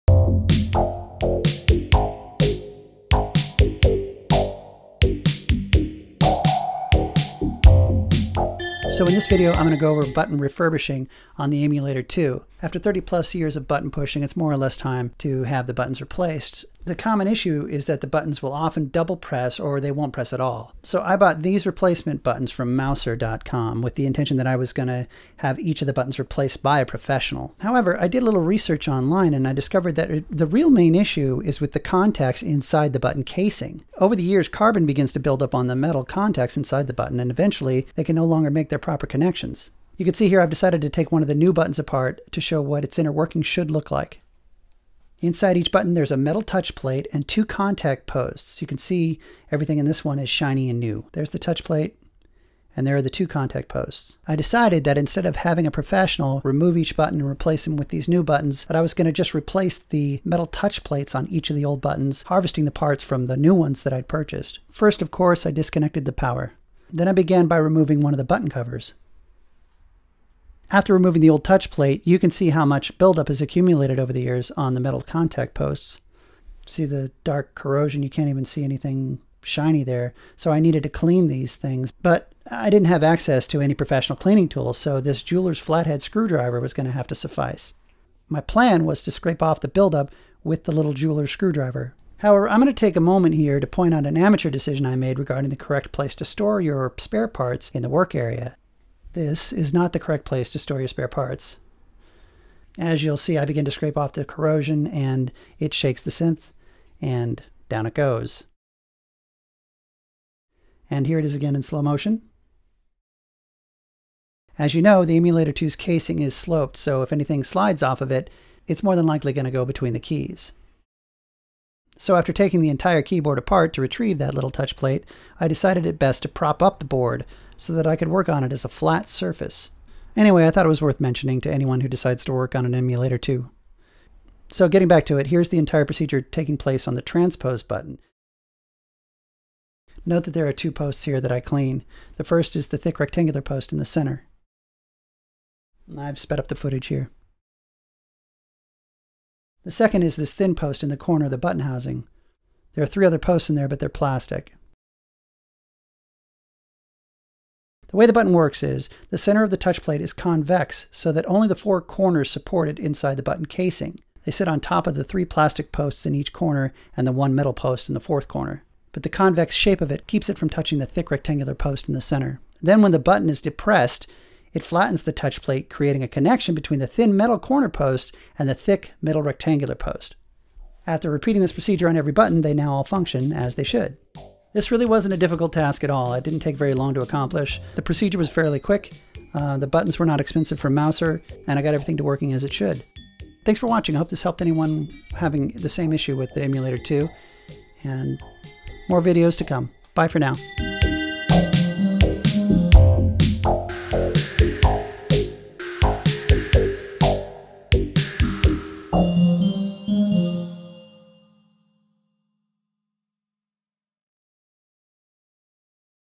EII Button Refurbishing
In this video, I refurbish all the buttons on the EII.